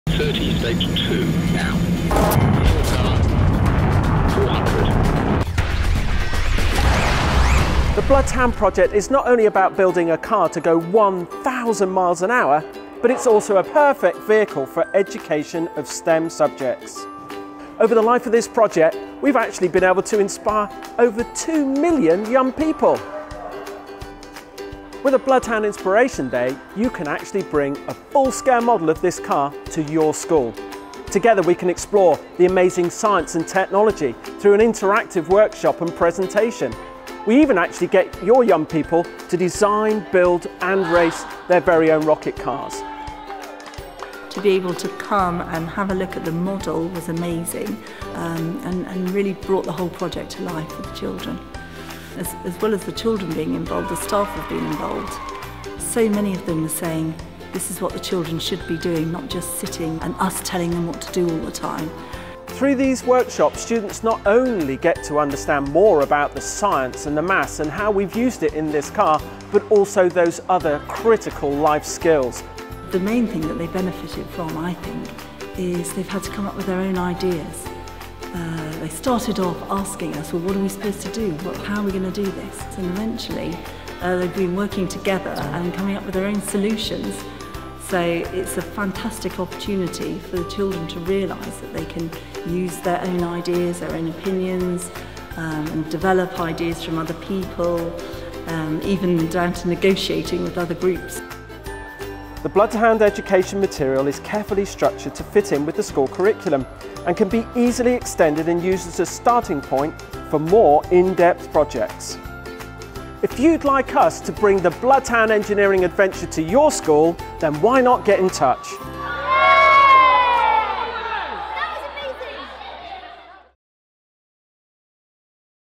BLOODHOUND Education
The BLOODHOUND Project is not only about building a car to go 1,000mph, but it is also a perfect vehicle for education of STEM subjects. Over the life of this project we've been able to inspire over 2 million young people!